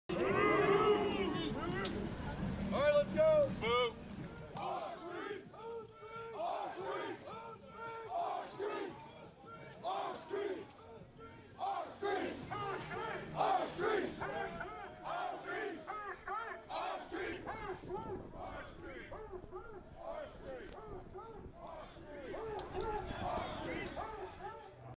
Trump Supporters March in Washington, D.C., USA, 2021
The morning after the notorious 2021 US Capitol Riots, pro-Trump advocates take to the streets of Washington DC as they continue to show their opposition to the 2020 US Presidential results.